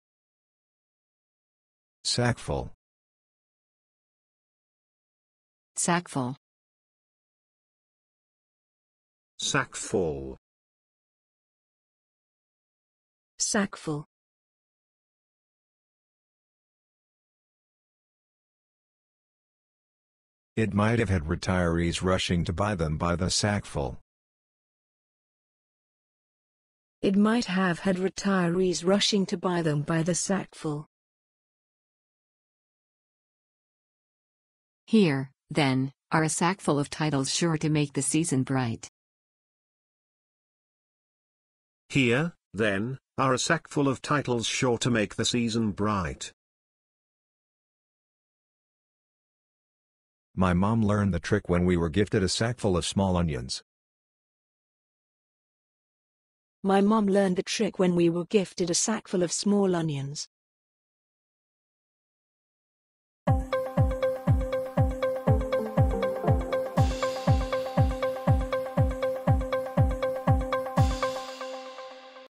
SACKFUL pronunciation | Improve your language with bab.la
Improve your spoken English by listening to SACKFUL pronounced by different speakers – and in example sentences too. Learn and love languages with bab.la.